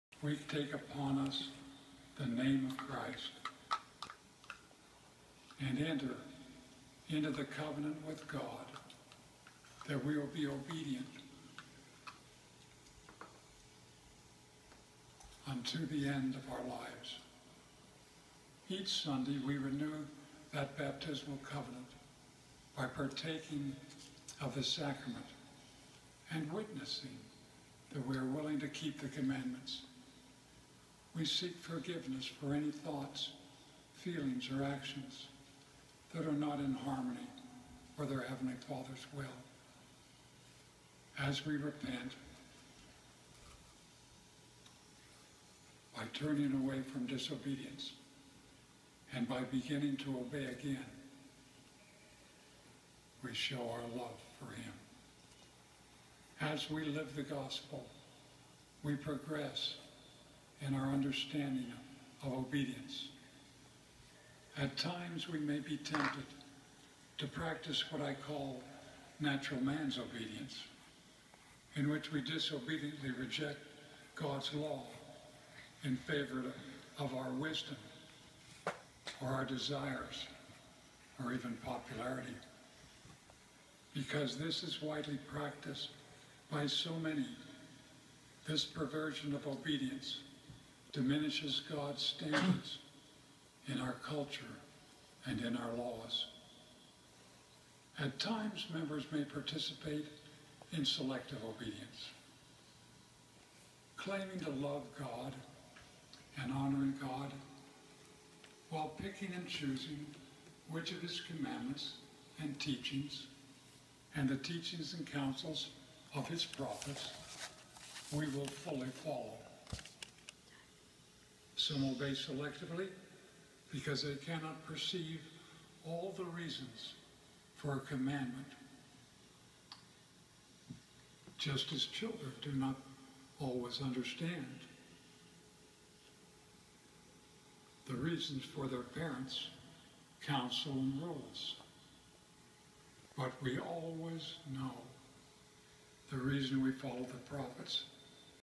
Creative Drawing Samples
Watch 15 year-old Scott work on his talent during conference.  He is excellent at making little animals and creatures.